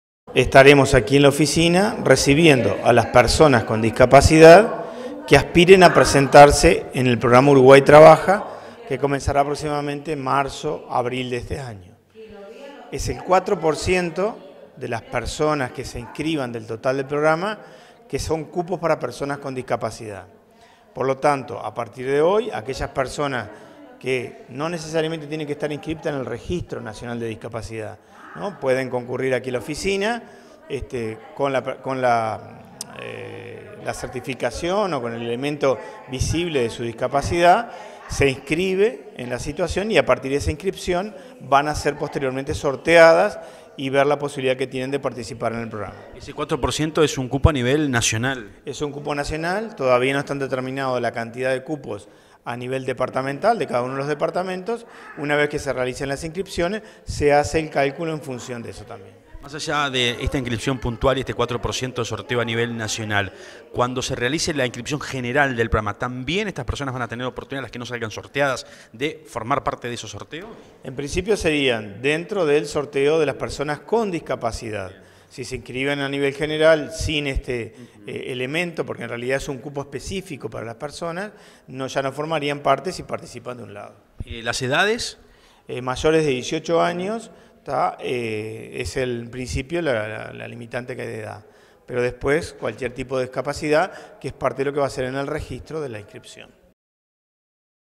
PROGRAMA URUGUAY TRABAJA/Abierto período de inscripciones
DR. RICARDO FLEITAS / Jefe Oficina Territorial Mides
ÚNICAMENTE PARA PERSONAS CON DISCPACIDAD/ Desde hoy y hasta el 7 de febrero
Desde hoy miércoles y hasta el martes 7 de febrero, el programa Uruguay trabaja del mides, habilitó el período de inscripciones únicamente para personas con discapacidad.